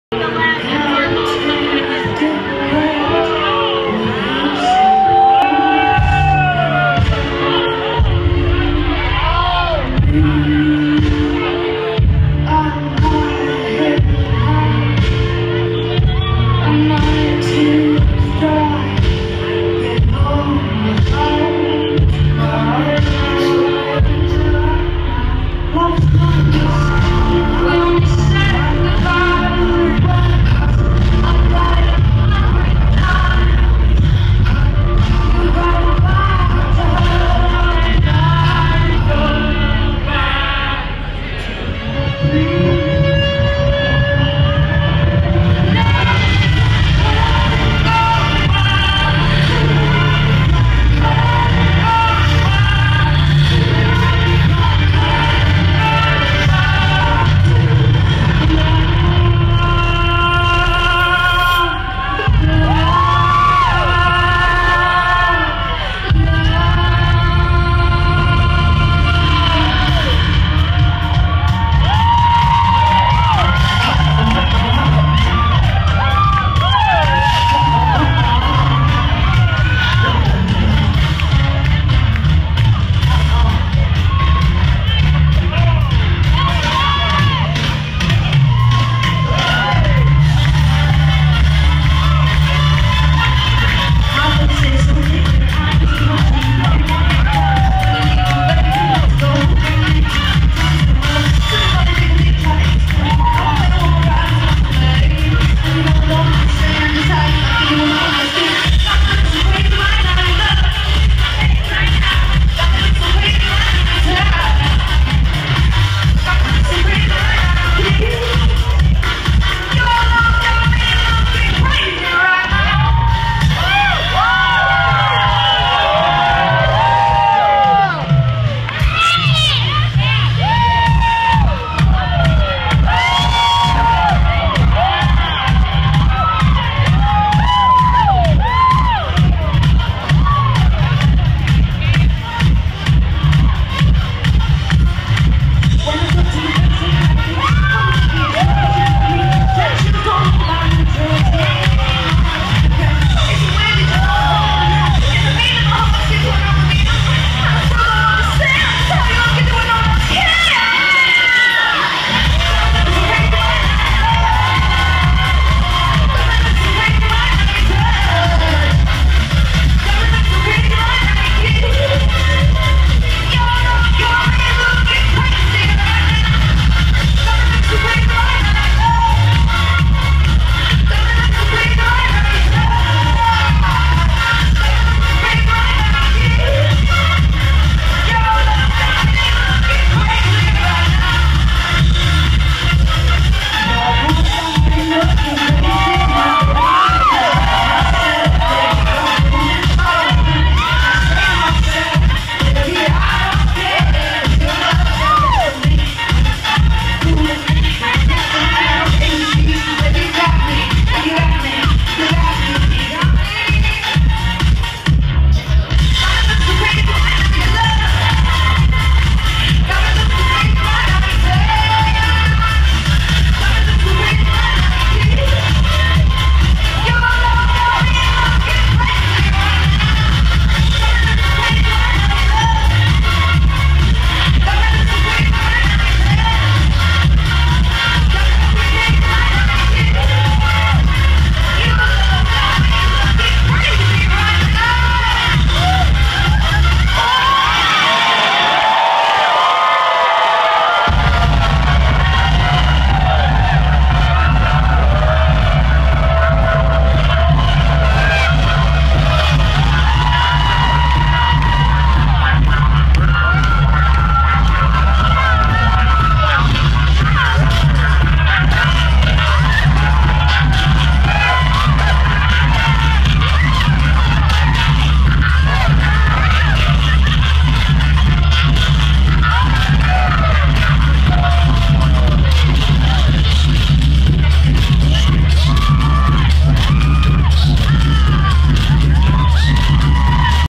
Closing Ballroom Performance at The Great Gatsby Superciema Party
Closing ballroom performance (featuring Kat Cunning) at the McKittrick Hotel's Great Gatsby Supercinema Party on 5/21.